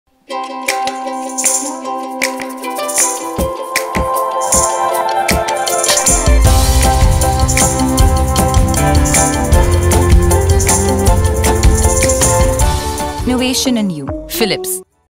Philips Hair Dryers | Don’t let them hang wet. Dry with total protection!
Your wet hair are prone to issues such as hair fall, split ends or dandruff etc. So don’t let your hair hang wet, dry them with total protection!. Philips Hair Dryers with ThermoProtect technology minimize the heat damage and gives your hair the care they deserve.